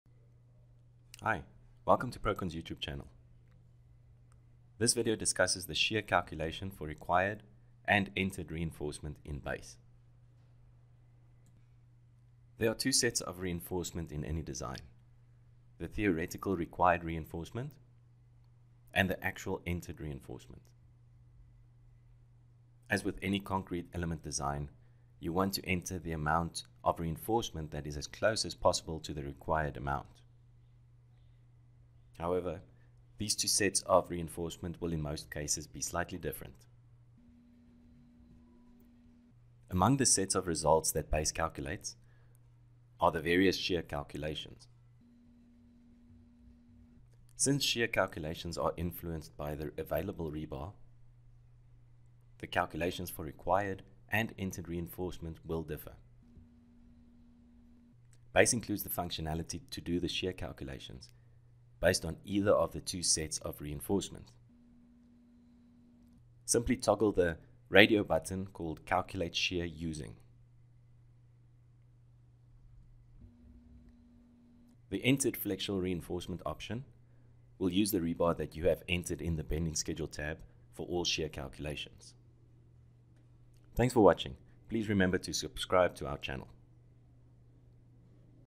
Required vs Entered reinforcement in Base
The video explains PROKON's Base module's functionality of doing the shear calculations with entered reinforcement.